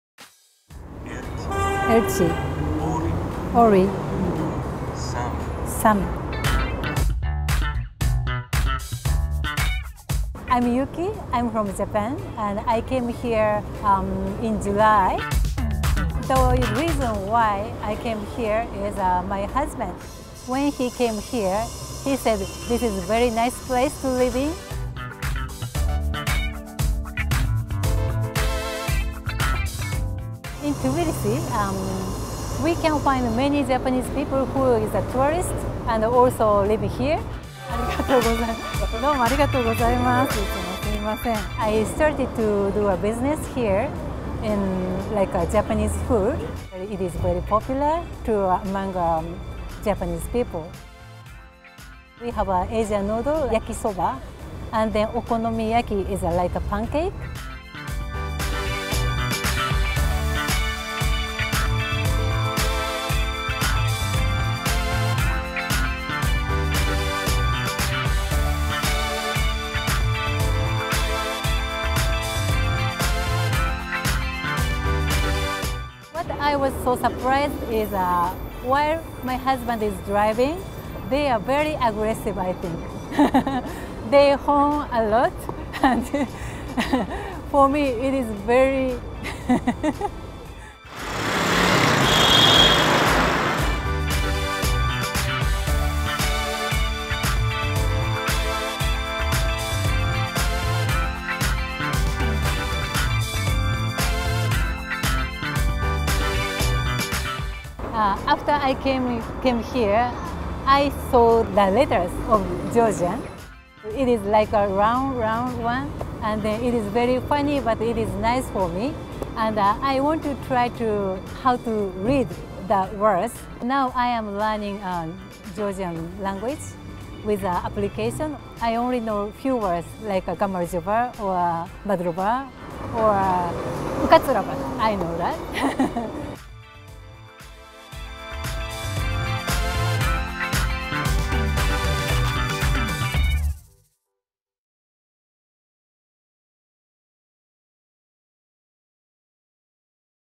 იუკის ოკონომიაკები თბილისურად
იუკი იტო სულ რაღაც 3 თვეა, რაც თბილისში ცხოვრობს, მაგრამ უკვე თავისი საქმე წამოიწყო.  ვერაზე ქუჩის ტრადიციულ იაპონურ კერძებს ამზადებს.  მხოლოდ იაპონელებისთვის ჩვეული თავაზიანობით ბევრის - პირველ რიგში კი, საქართველოში ჩამოსული თანამემამულეების - ყურადღებას  იპყრობს.